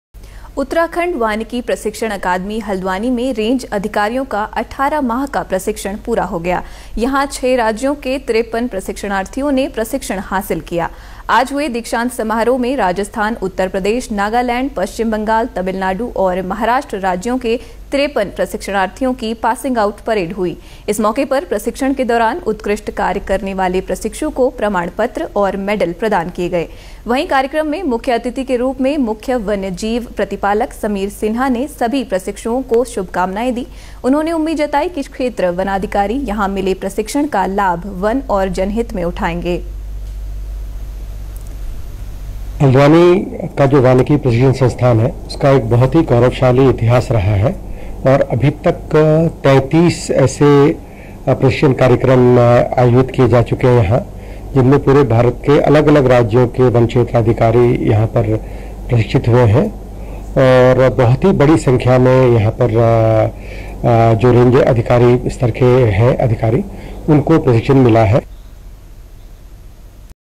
उत्तराखंड वानिकी प्रशिक्षण अकादमी हलद्वानी में रेंज अधिकारियों का 18 माह का प्रशिक्षण पूरा
उत्तराखंड वानिकी प्रशिक्षण अकादमी हलद्वानी में रेंज अधिकारियों का 18 माह का प्रशिक्षण पूरा हो गया। यहां 6 राज्यों के 53 प्रशिक्षणार्थियों  ने प्रशिक्षण हासिल किया। आज हुए दीक्षांत समारोह में राजस्थान, उत्तर प्रदेश, नागालैंड, पश्चिम बंगाल, तमिलनाडु और महाराष्ट्र राज्यों के 53 प्रशिक्षणरार्थियों की पासिंग आउट परेड हुई।